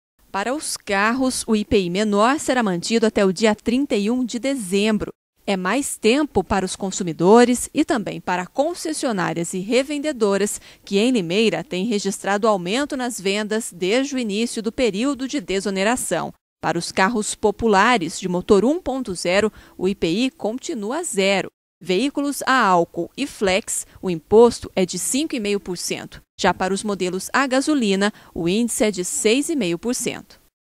Prorrogada a redução do IPI para automóveis
Governo federal mantém desoneração até 31 de dezembro